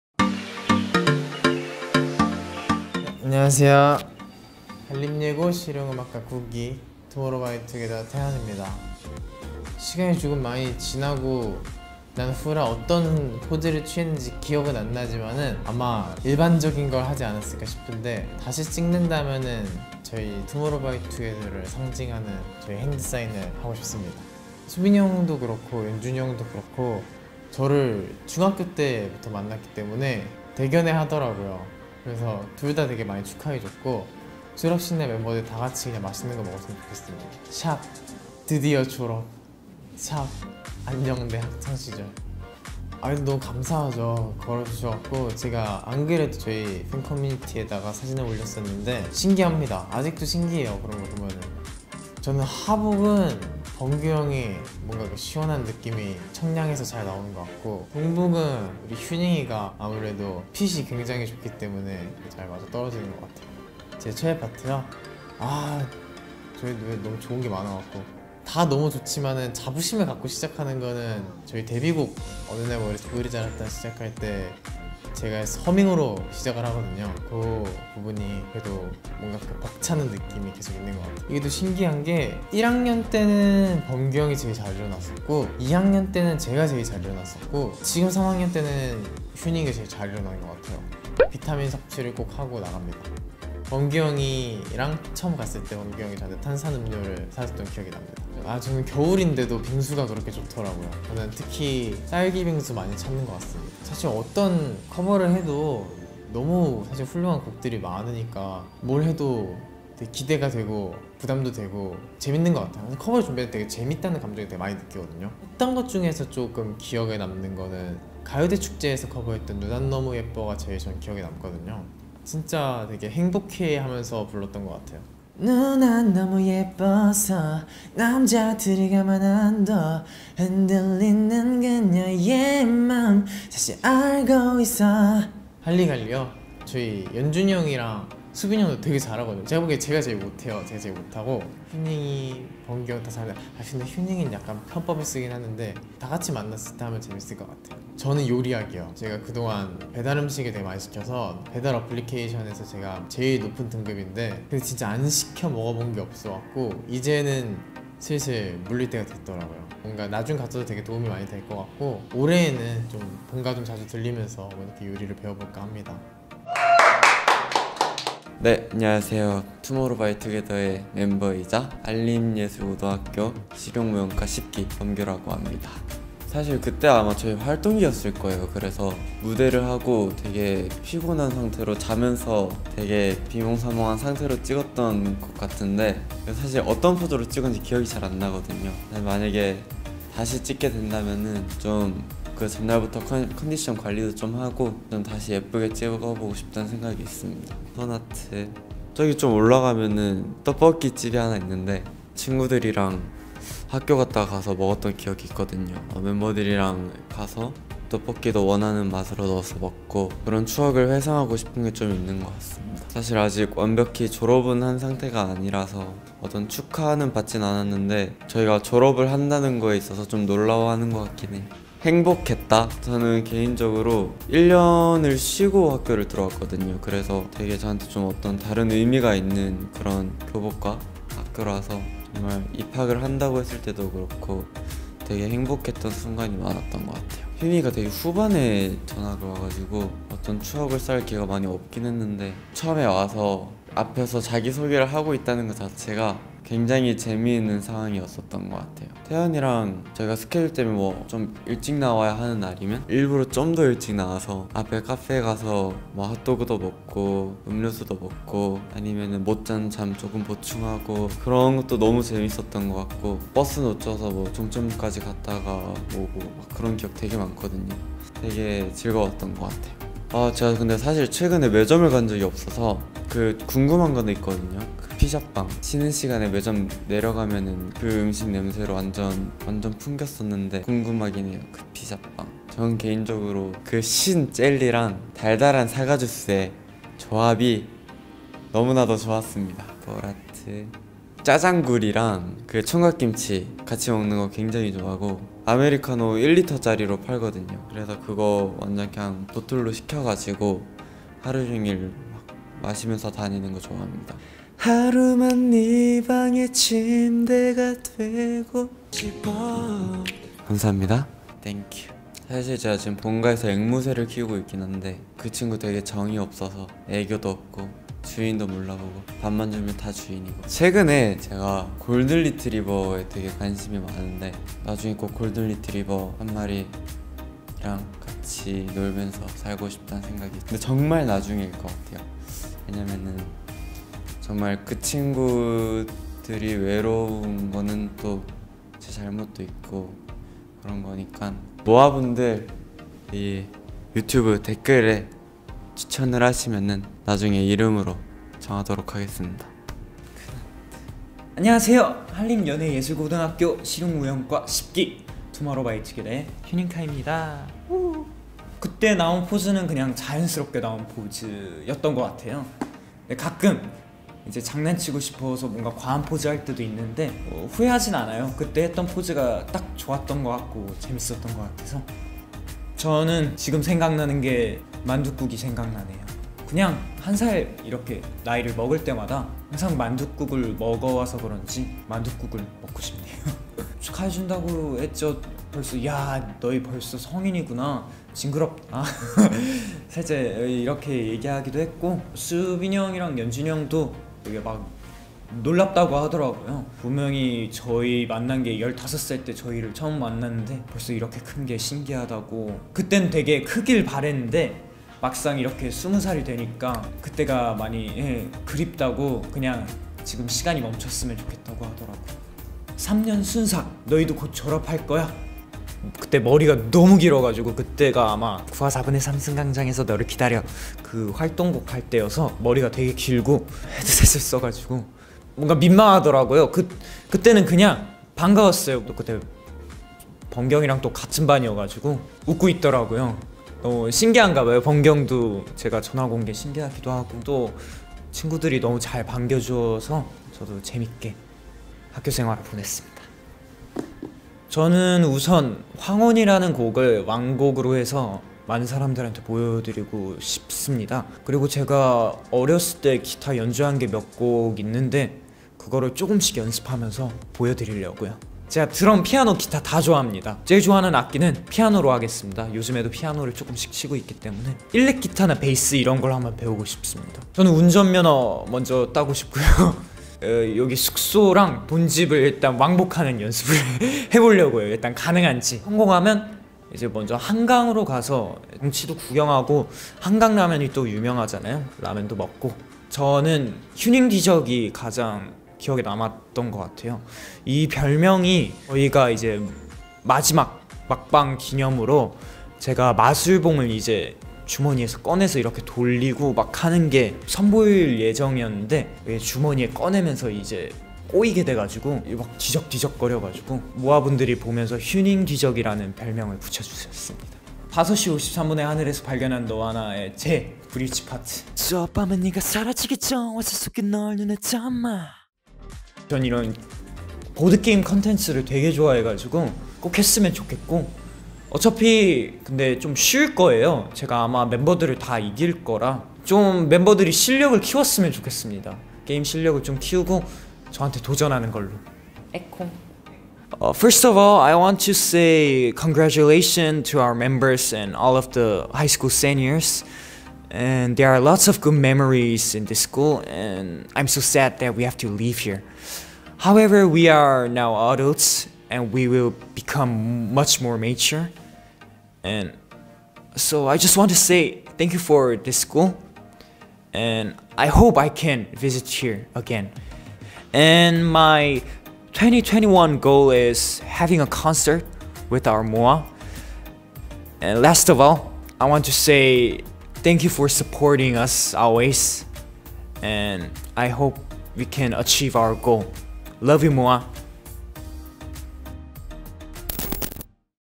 [ENG/INDO SUB] 한림예고 졸업 인터뷰🎓 - 투모로우바이투게더
ENGLISH, INDONESIA, JAPAN SUBTITLE AVAILABLE~~

투모로우바이투게더 한림즈 졸업합니다!!🎉🎉🎉 올해 졸업하는 범규·휴닝카이(실용무용과 10기), 태현(실용음악과 9기)이가 한 자리에 모였어요! 🤗 교복 입은 한림즈 모습은 그저 ✨갓벽✨ 학교 생활 썰도 풀고🏫 노래도 부르고🎶 인터뷰하는 모든 순간이 귀여운 한림즈! 다음 주엔 투모로우바이투게더 졸업 인터뷰 2탄이 업로드 된다는 사실까지! 기대해주세요~ (한림즈에게 보내는 졸업 축하 메시지 & 미래의 범규 애완동물 이름! 댓글로 많이 남겨주실거죠?😉) ***촬영에 협조해주신 빅히트 엔터테인먼트 관계자 여러분께 감사드립니다🙏🏻 ※